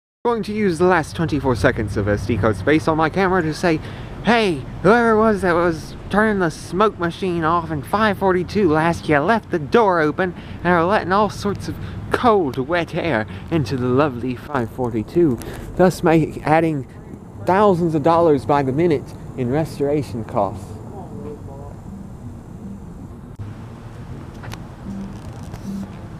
Another Mistake Adding Another Million To The Restoration Costs Of A Steam Locomotive
Of course it is still being treated better than GSMR trying to see how many parts on 722 they can expose to the elements. Please don't dislike, please don't unsubscribe.